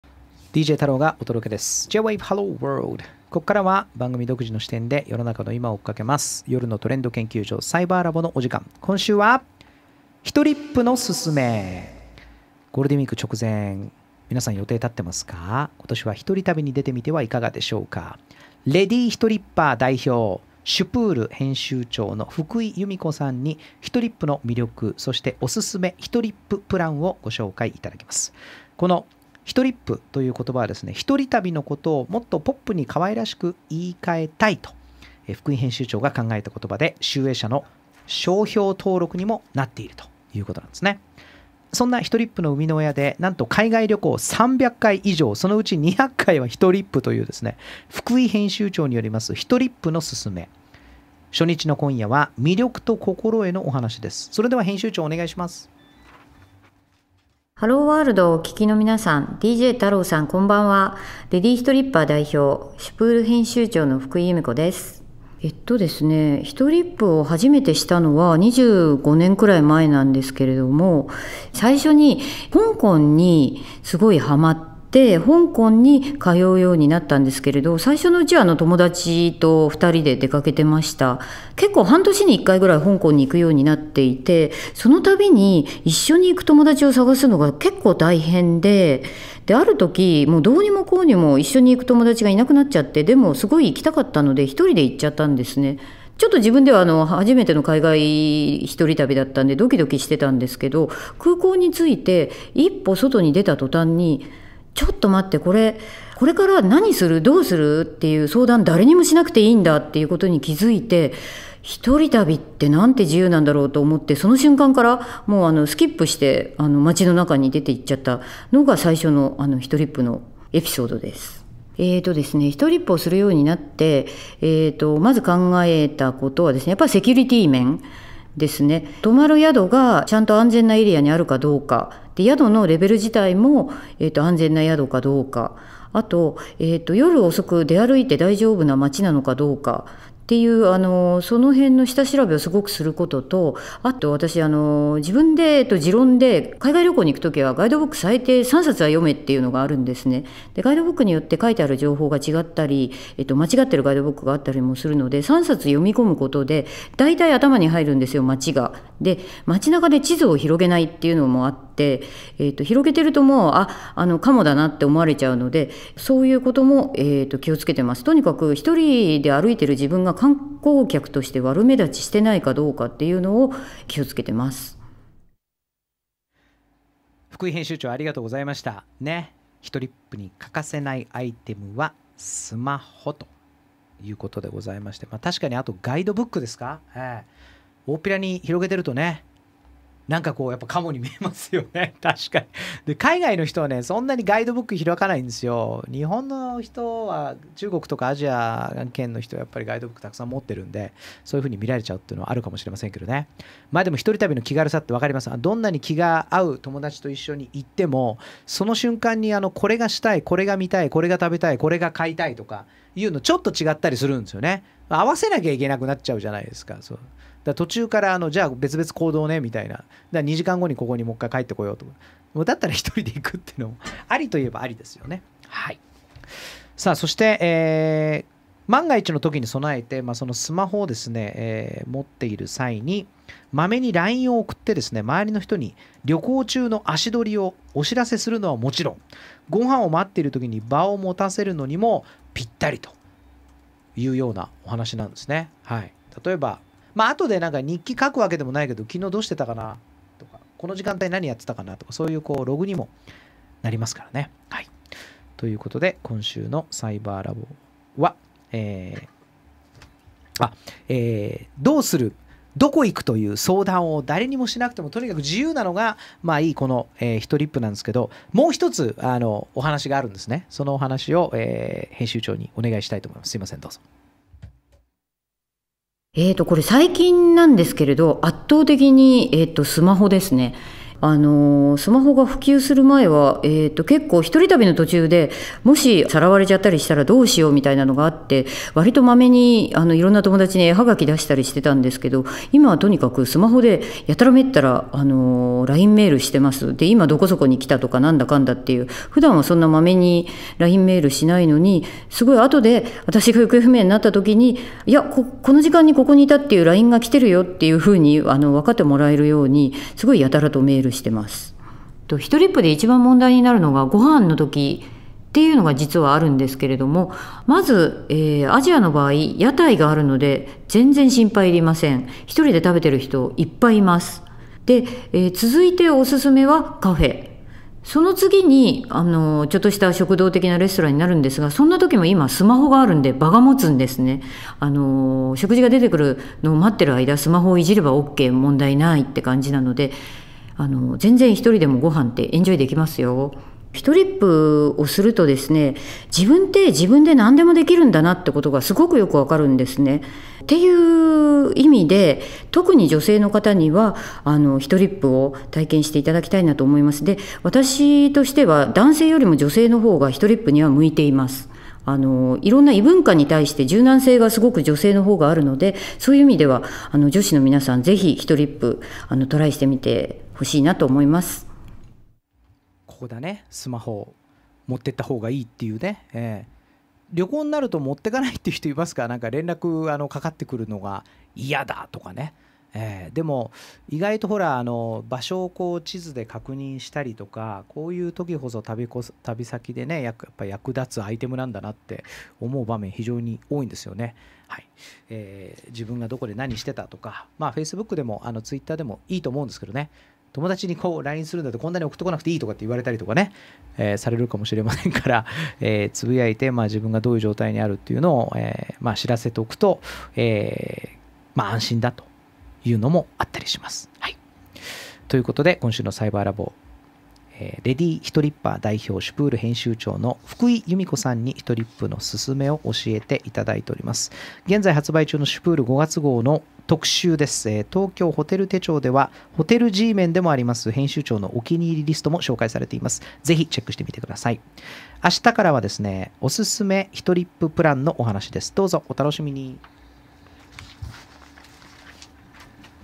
「ひとりっぷのすすめ」 -ひとりっぷの魅力とマストアイテム-
今週はレディひとりっぱー代表！
SPUR（シュプール）編集長の福井由美子さんに、
「ひとりっぷ」の魅力、そしておすすめプランをご紹介いただきます！
今夜は「ひとりっぷ」の魅力と心得、
そして欠かせないアイテムについてのお話です。